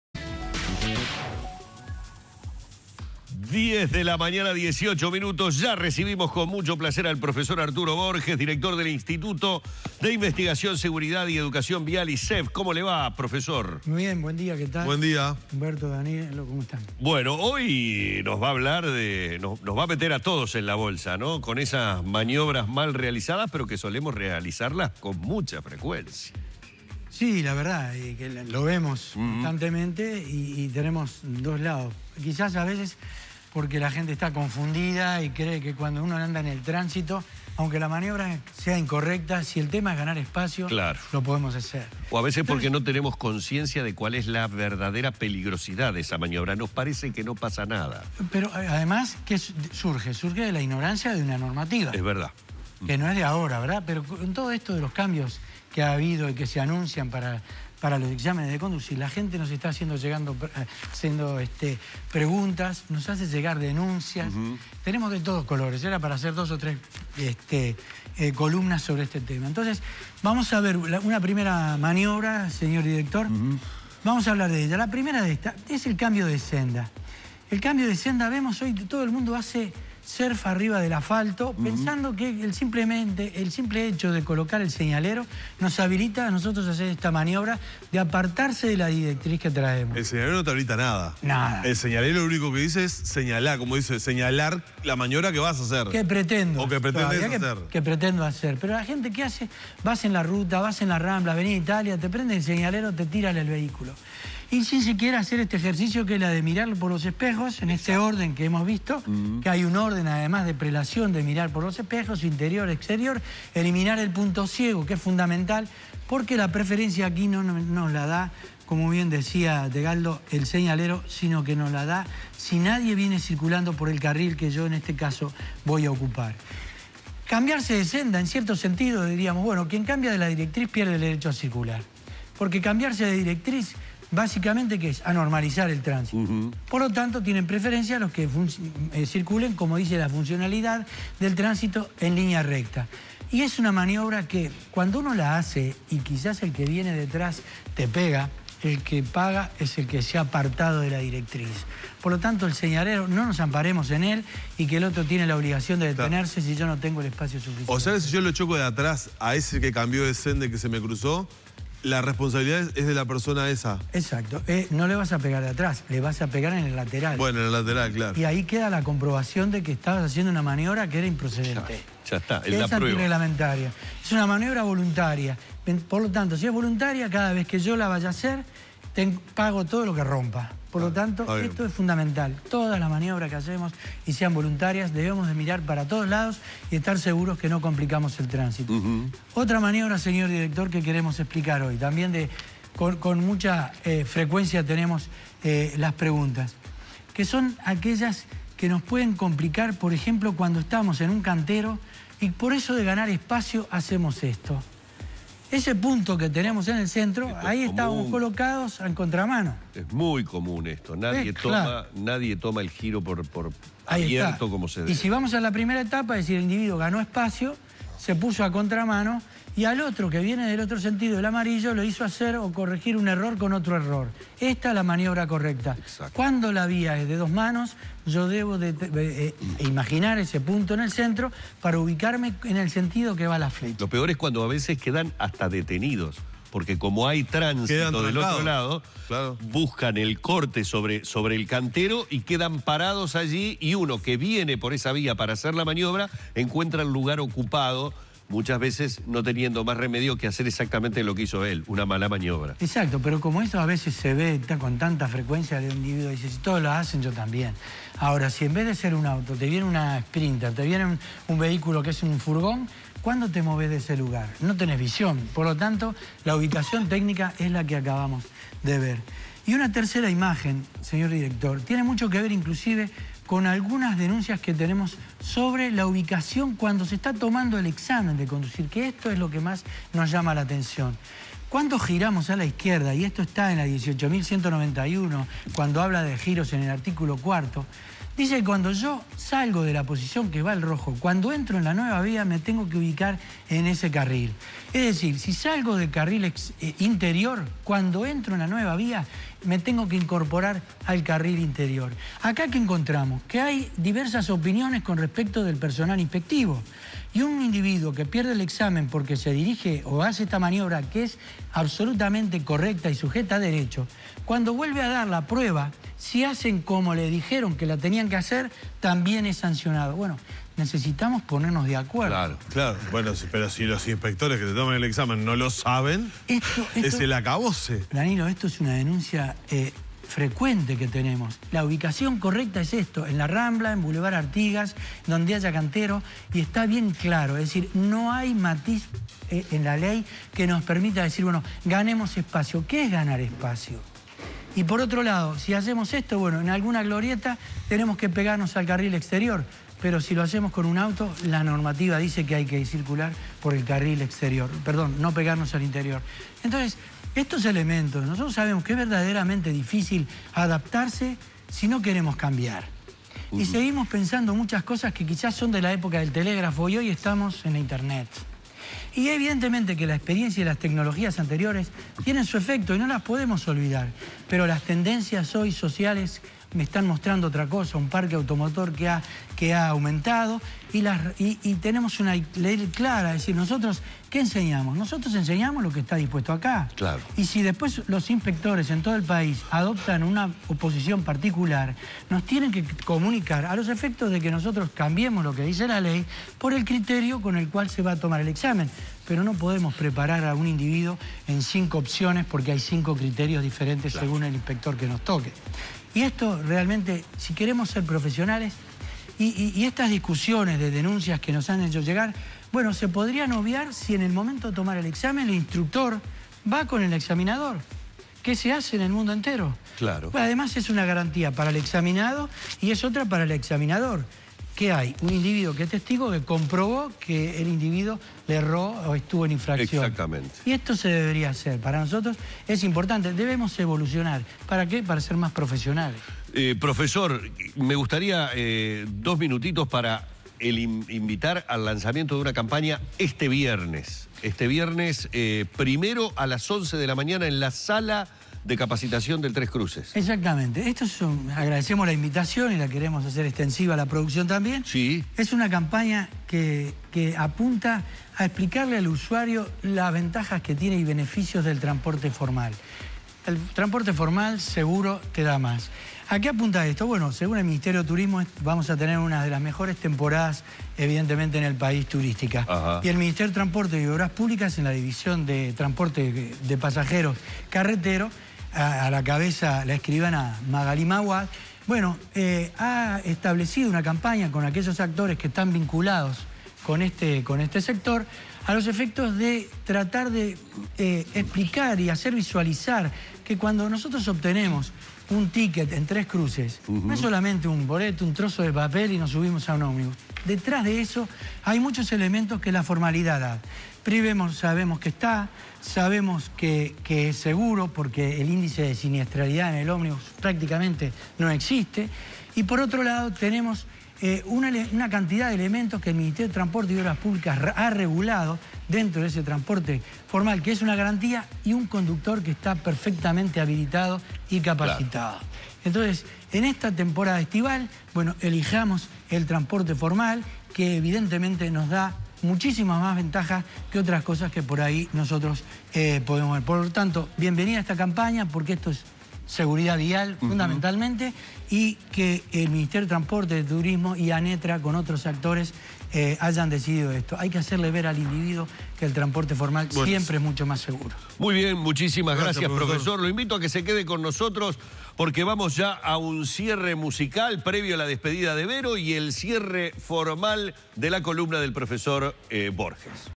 Arturo Borges - Seguridad Vial
Hoy en la columna de seguridad vial hablamos de ciertas maniobras que se ven mal realizadas con frecuencia. Para eso recibimos al Profesor Arturo Borges, Director del Instituto de Investigación, Seguridad y Educación Vial, ISEV. (Centro de Formación de Conductores para el Transporte y la seguridad Vial con 20 años en la plaza, que cuenta con la Asistencia Técnica de la Universidad de Valencia.